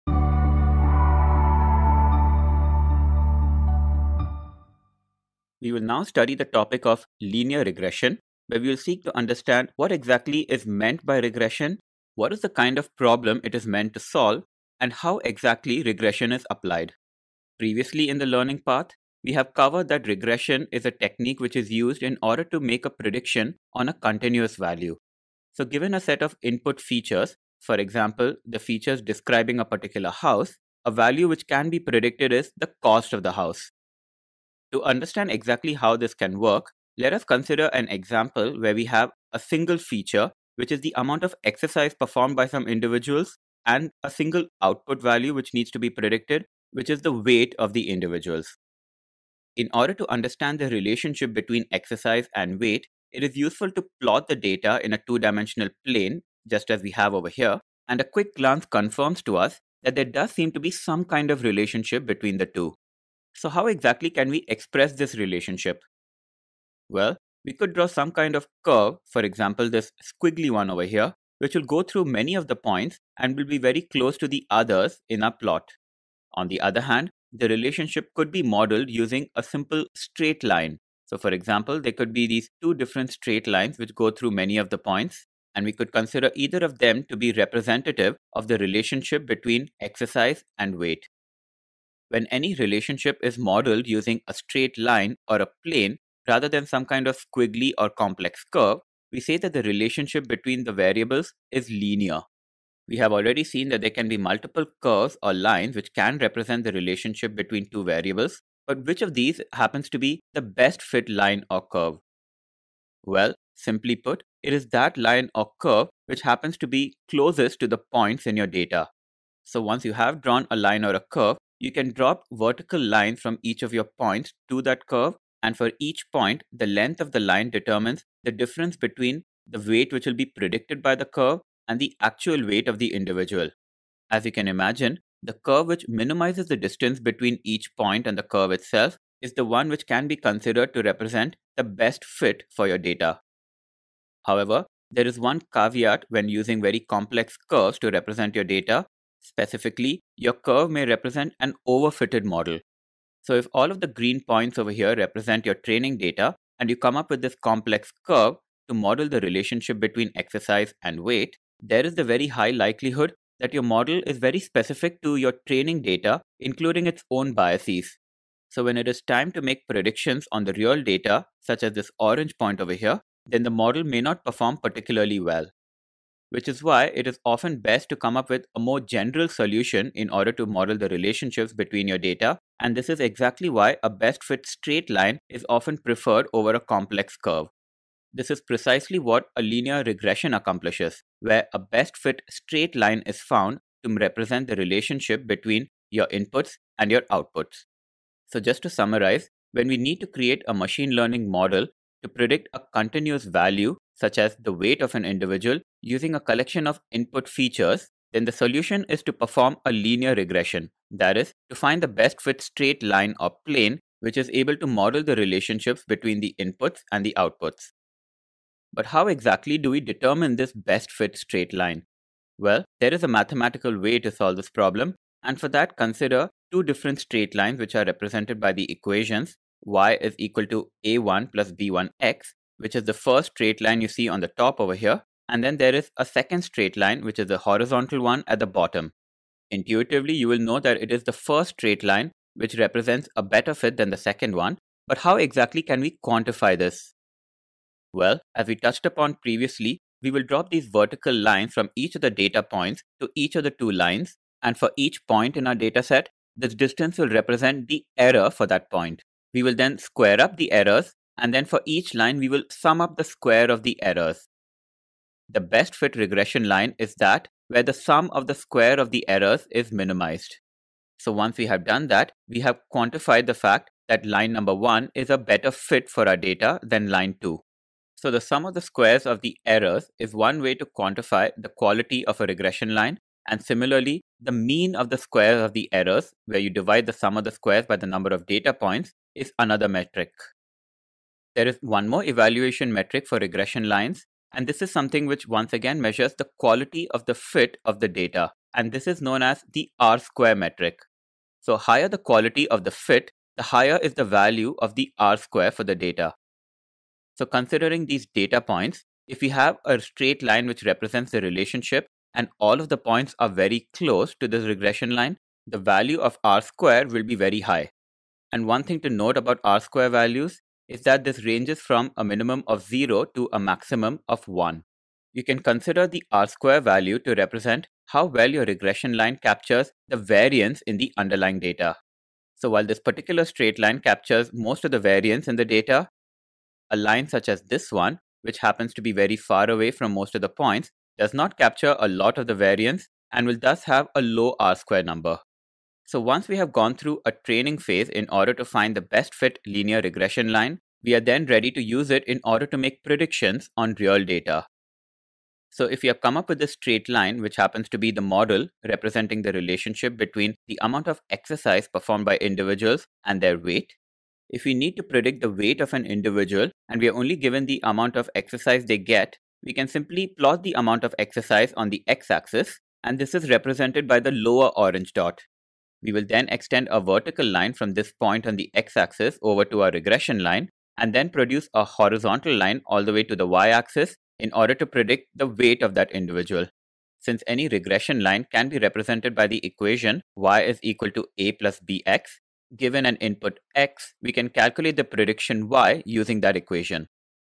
Introduction to Linear Regression - Machine Learning Explained!
We'll explore the foundational concept of linear regression in machine learning. Learn how linear regression models the relationship between variables, understand how it finds the best fit straight line, and explore its application in solving real-world problems. Whether you're a data science beginner or looking to deepen your knowledge, this video provides a clear and comprehensive overview.

Machine learning is the field of computer science that gives computer systems the ability to learn from data — and it's one of the hottest topics in the industry right now.

What You'll Learn In This Video:
- Linear Regression Fundamentals: Discover what linear regression is and how it models relationships in data.
- Best Fit Line: Understand how linear regression finds the best fit straight line to represent data relationships.
- Regression Applications: Gain insight into the practical applications of regression and the types of problems it solves.